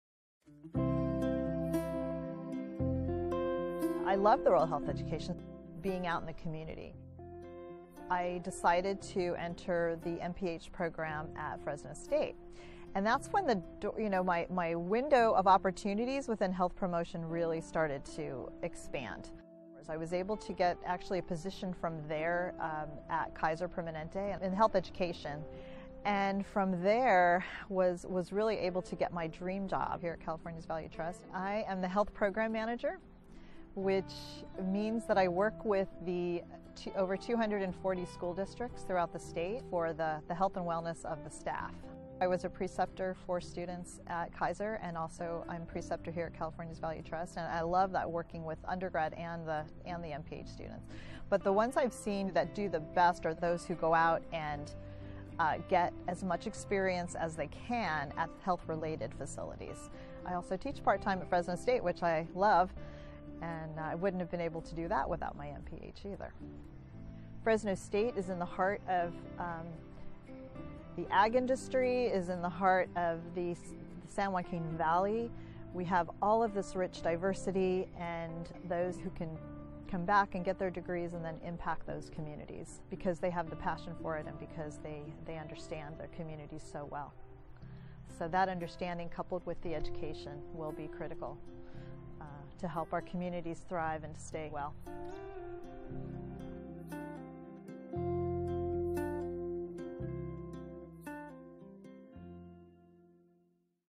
Career Profile: Robin Wood, MPH
Through our two-year Master of Public Health Program at Fresno State, a wide range of professional opportunities in the field are available. MPH alumna, Robin Wood, shares what she's been up to since graduating, which includes teaching right here at Fresno State!